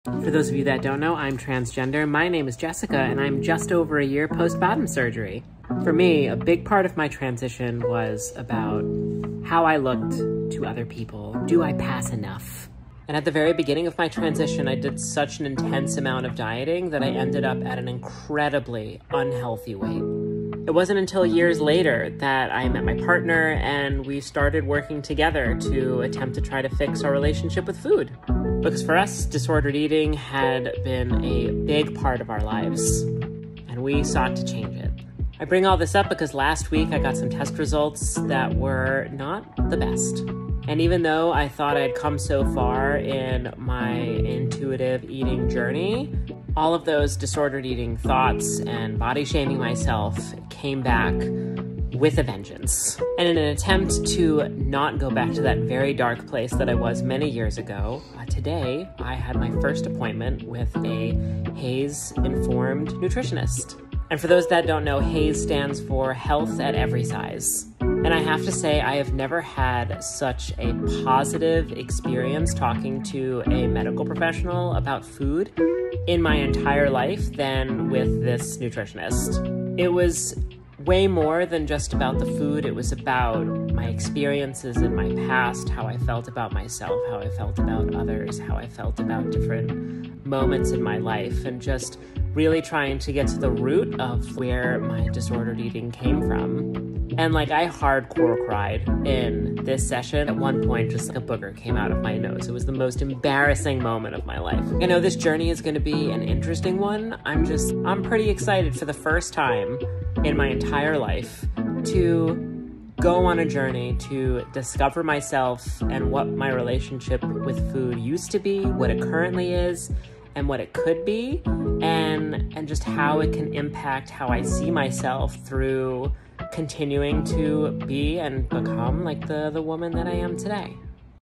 My Transition and Tenuous Journey with Food
My journey with food has been so hard and I’m so happy with how far I’ve come. This first session made me realize I still have work to do but I’m in such a good place to get that work done.